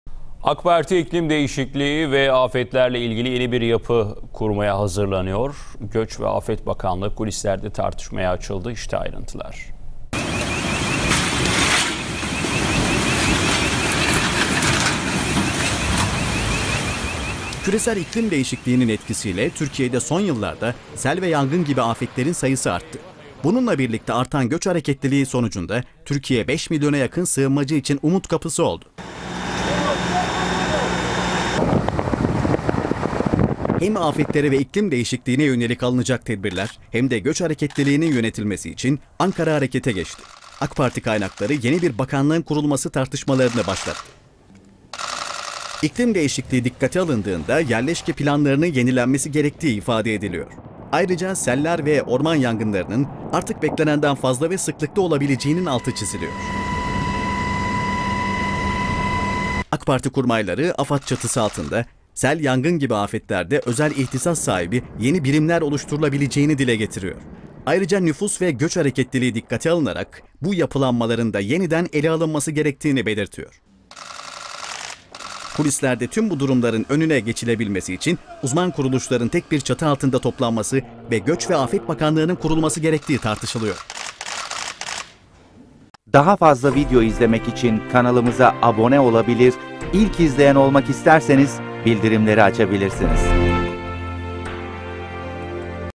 Göç ve Afet Bakanlığı'nın kurulması tartışmaya açıldı
Global iklim değişikliğinin etkisiyle Türkiye’de son yıllarda artan sel ve yangın gibi afetlerin yanı sıra, göç hareketliliğinin yönetilmesi ve güvenli yerleşim alanlarının oluşturulmasına yönelik planlamaların yapılması için yeni bir yapı kurulması tartışılıyor.

Türkiye’de 4 milyona yakın Suriyeli olmak üzere 5 milyondan fazla sığınmacı bulunuyor. Göç hareketleri İçişleri Bakanlığı bünyesindeki Göç İdaresi Genel Müdürlüğü ve AFAD tarafından yönetiliyor. AK Parti’de son dönemde hem bu göç hareketlerini hem de iklim değişikliği ile birlikte önümüzdeki yıllarda artacak insan hareketliliğinin yönetilmesi için yeni bir bakanlık kurulması tartışmaya açıldı. AK Parti kaynakları, sel, yangın ve deprem gibi afetlere hızlı müdahale, bu konularda ihtisaslaşmış yapıların tek bir çatı altında toplanması, tüm bunlarla birlikte göç hareketlerinin yönetimini de içine alacak şekilde bir bakanlık oluşturulmasını tartıştıklarını söyledi.